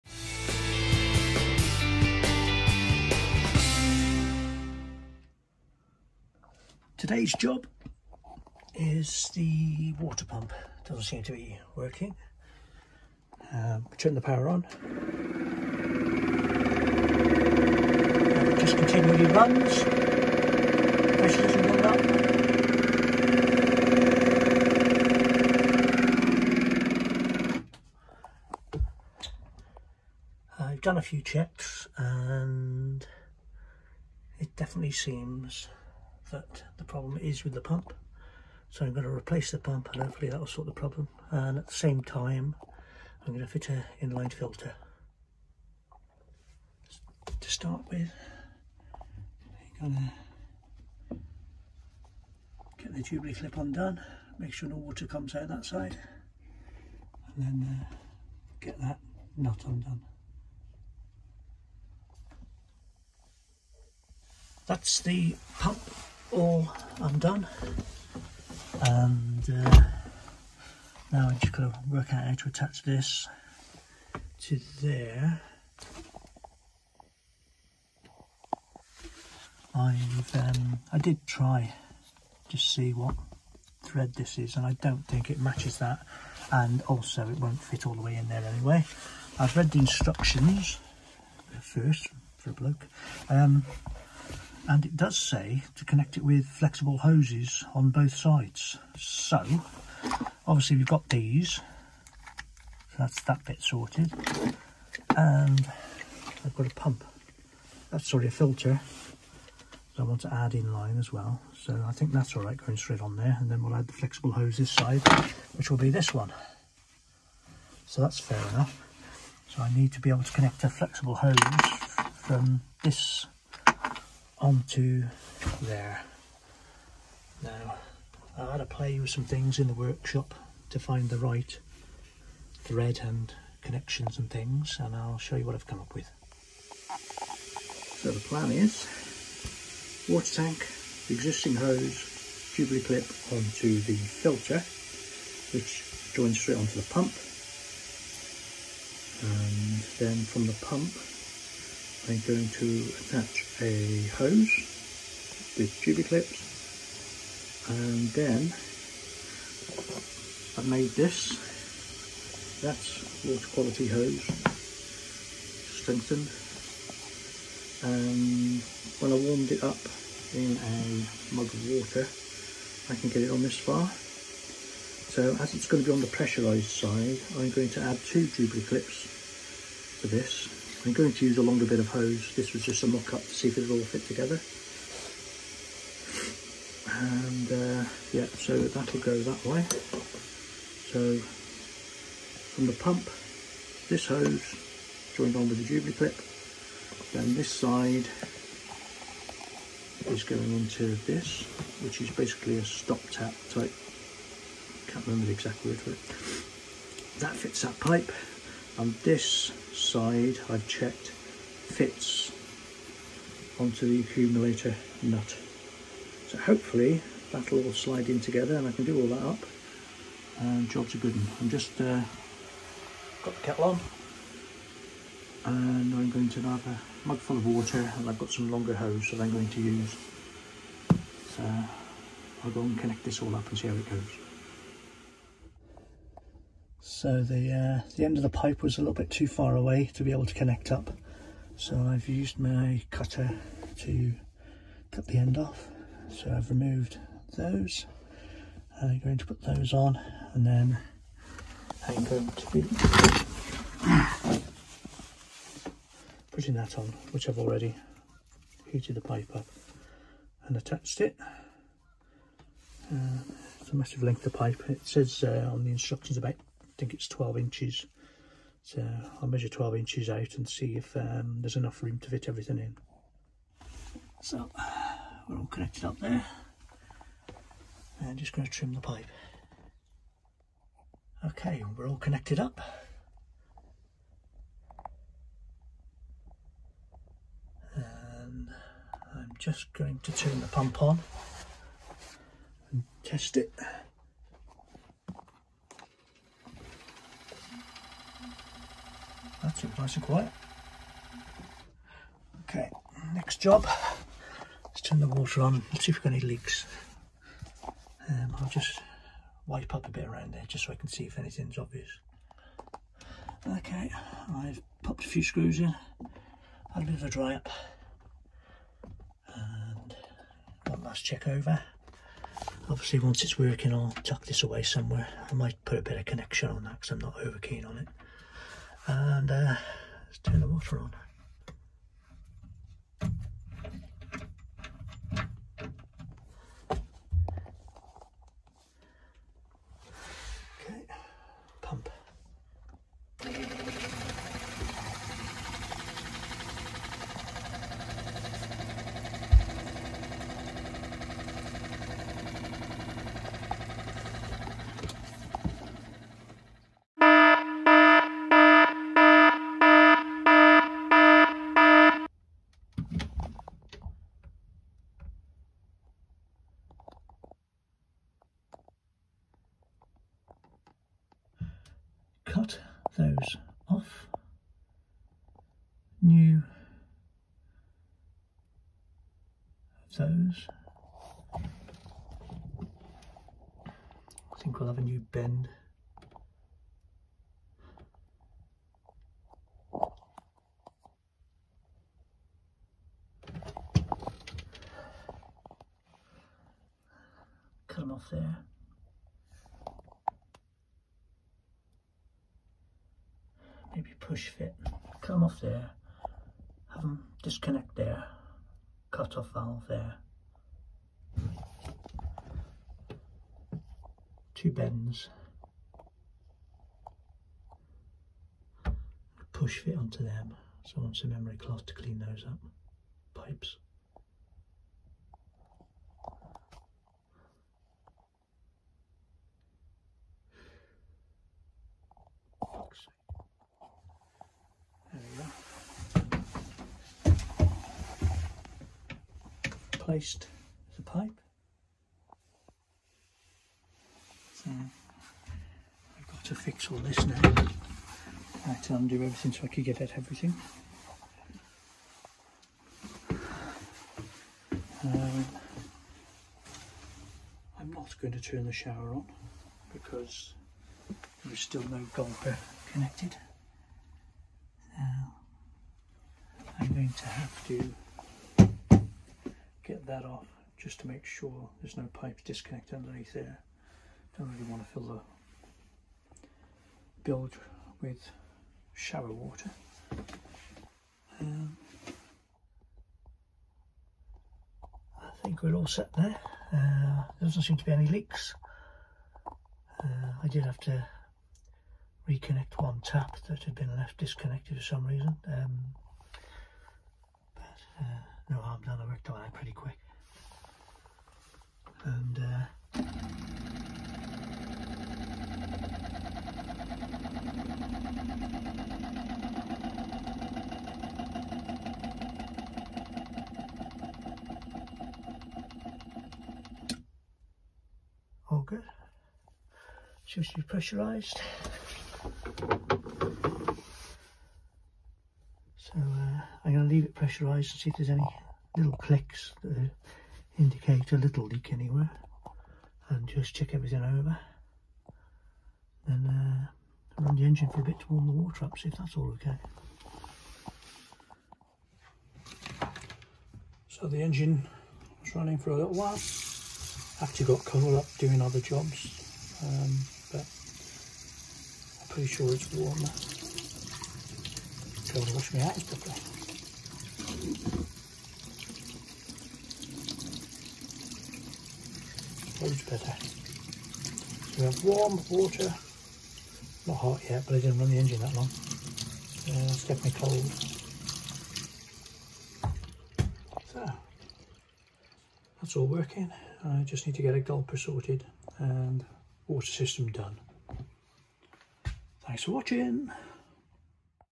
Narrowboat Project Part 4 - Water Pump Woes
Replacing the faulty water pump on my Project Narrowboat Renovation.
And forgetting to check all the pipes….